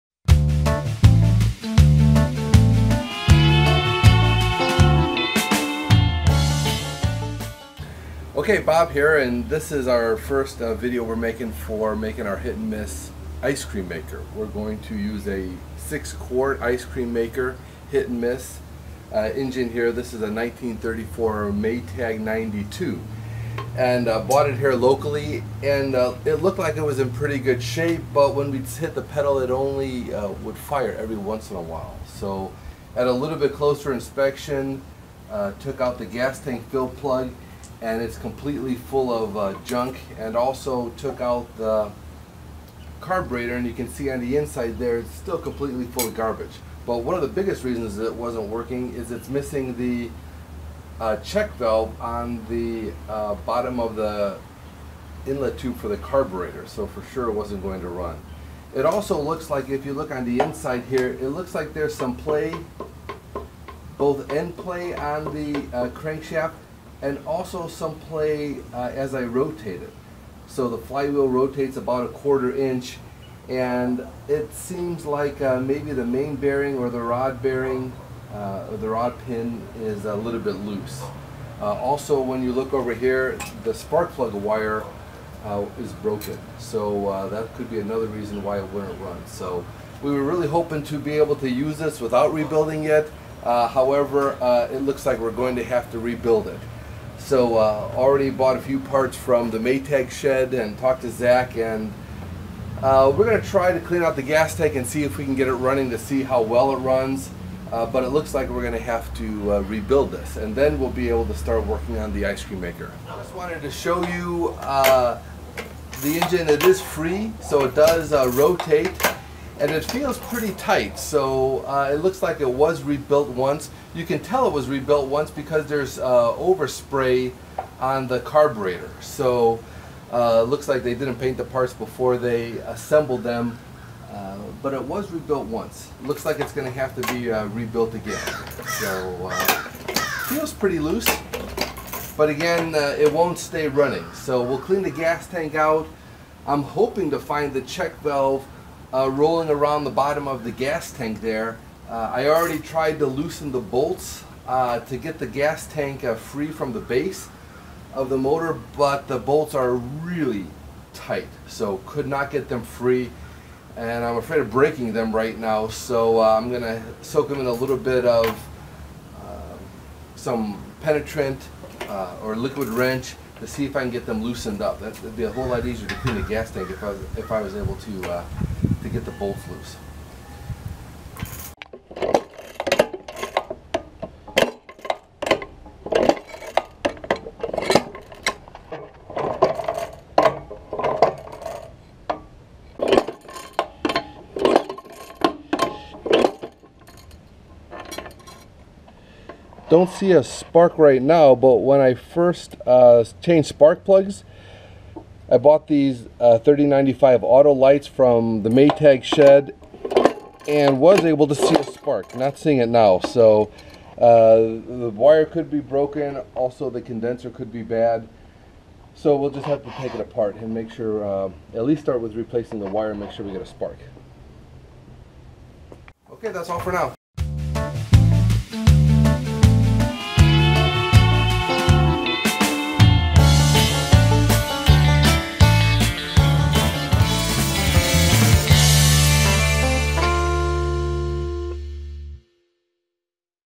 Ep.1- GETTING THE ENGINE TO RUN- Maytag 92 Hit and Miss Ice Cream Maker- MakerBob-
Building an ice cream maker using a 1934 Hit & Miss Maytag 92 engine.